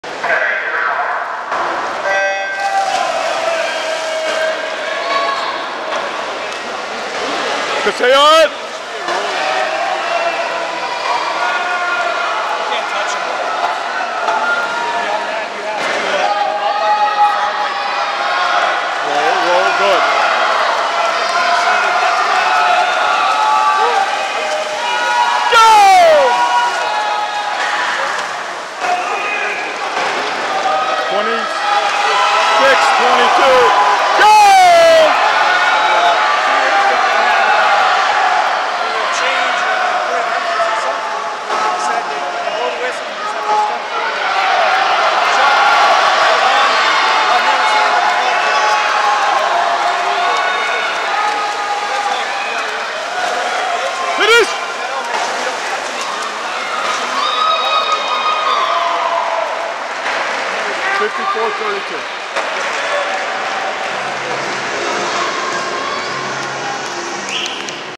Sead 54.32 in 100 back in PIAA Consol Finals
Sead swims 100 back in consol finals at 2015 PIAA Class AA Meet in 54.32. He beat his prelim time. PR from WPIAL meet.